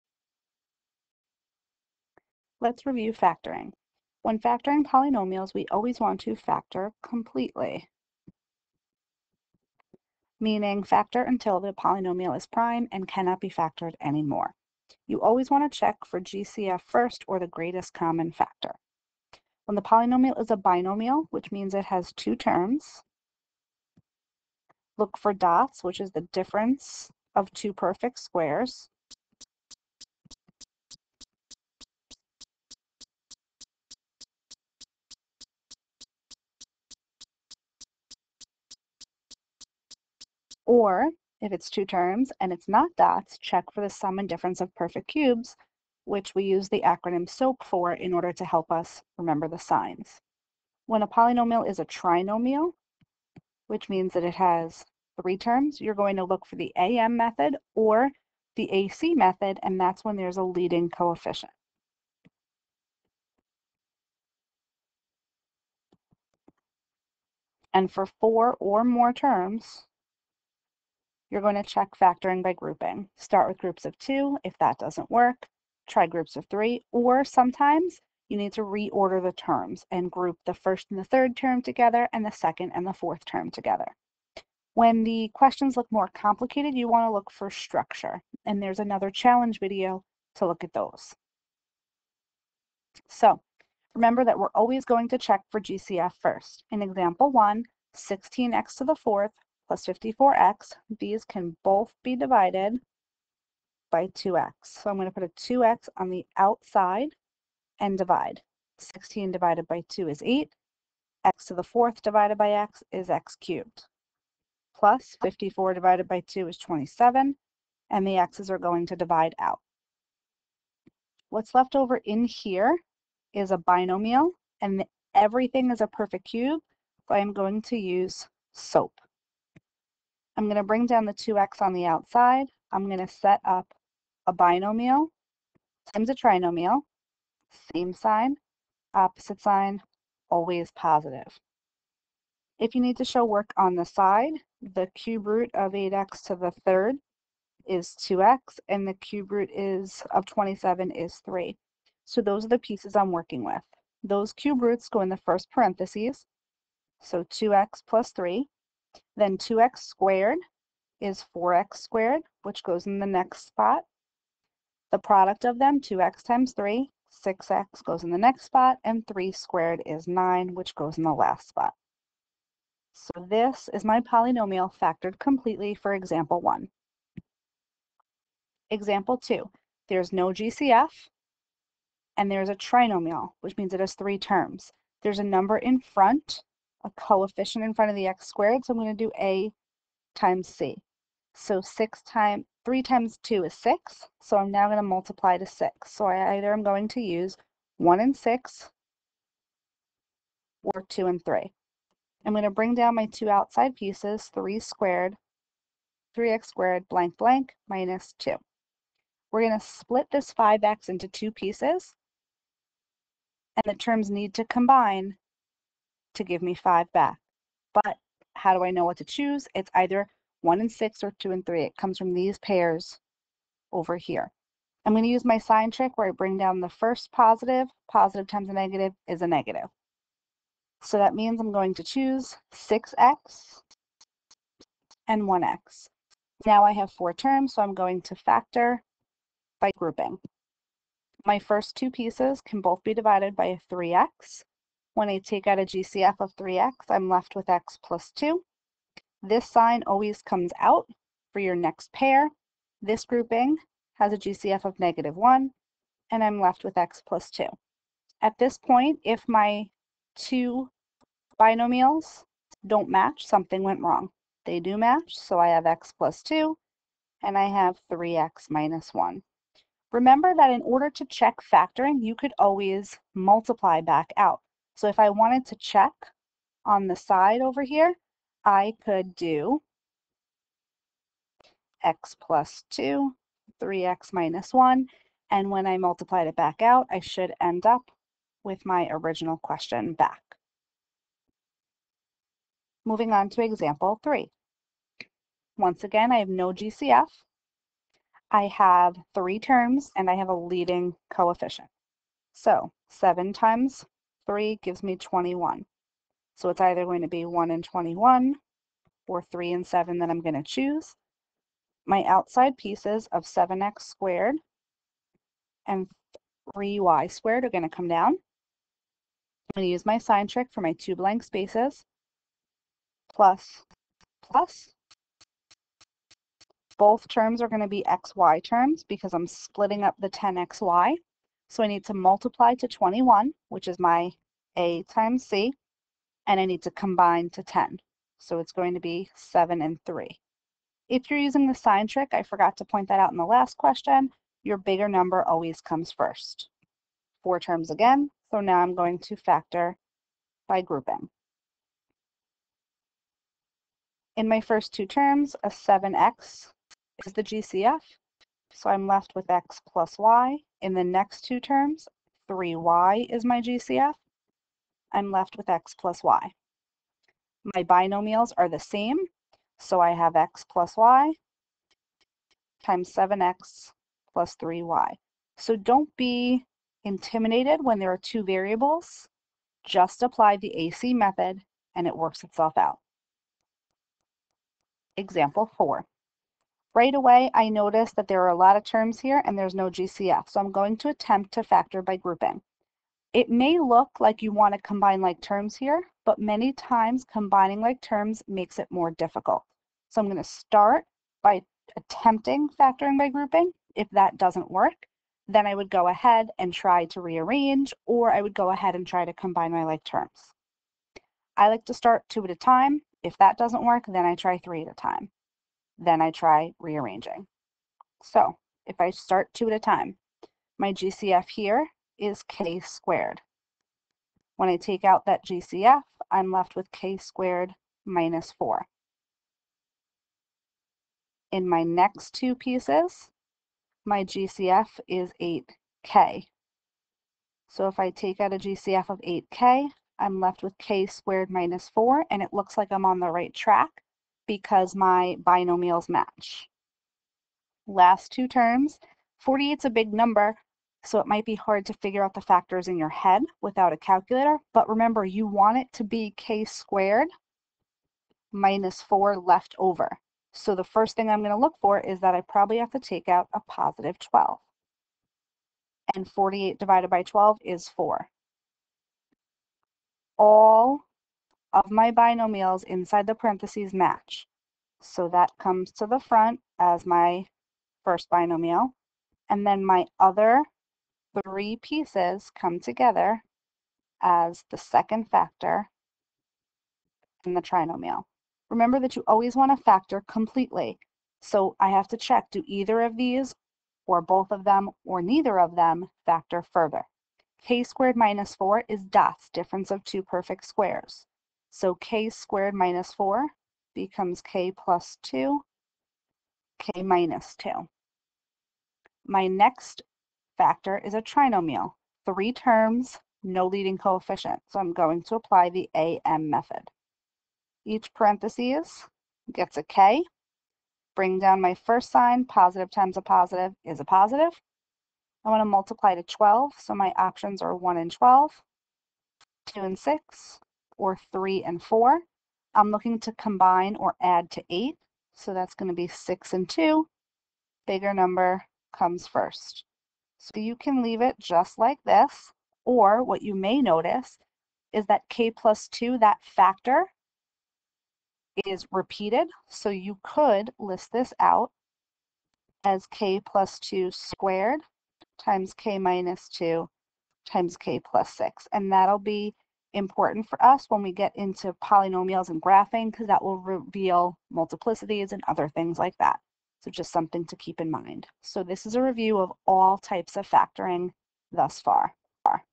Factoring review
This video reviews 4 different factoring questions which reviews GCF, DOTS, sum of perfect cubes, trinomials with and without a leading coefficient, trinomials with multiple variables, factoring by grouping and factoring completely